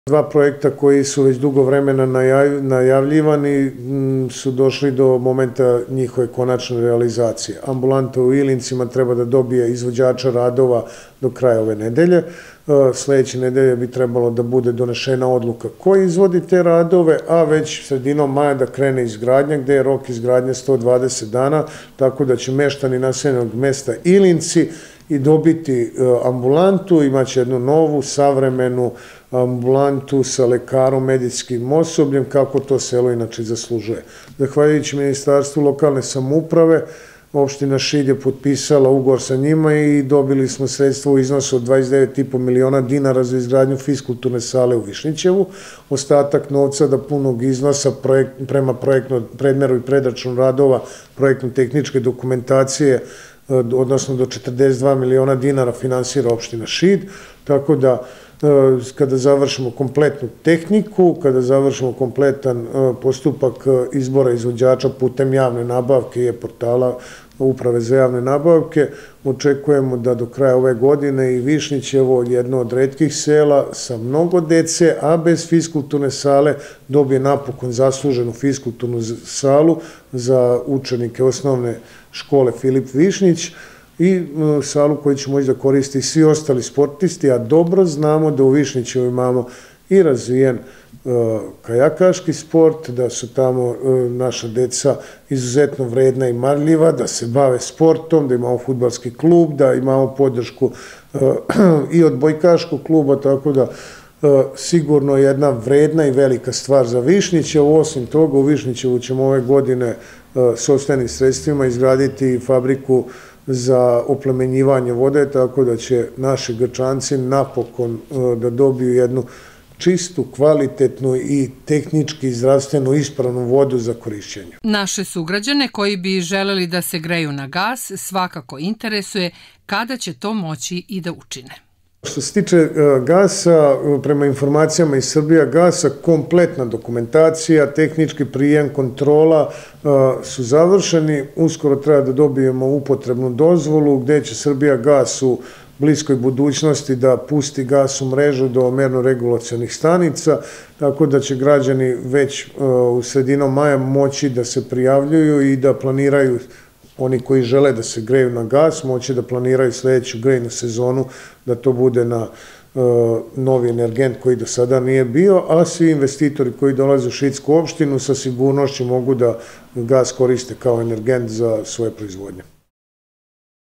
Najava dva projekta i gasifikacije
Na nedavno održanoj konferenciji za štampu predsednik Opštine Šid, Zoran Semenović upoznao je novinare sa dva važna projekta i gasifikacijom na teritoriji naše opštine. Sredinom maja trebalo bi da otpočne izgradnja zdravstvene ambulante u Ilincima. Drugi značajan projekat je izgradnja fiskulturne sale za potrebe Osnovne škole „Filip Višnjić” i sportista u Višnjićevu. Predsednik Semenović najavio je i početak prijavljivanja građana za gas, koje se očekuje sredinom maja, čim stigne upotrebna dozvola.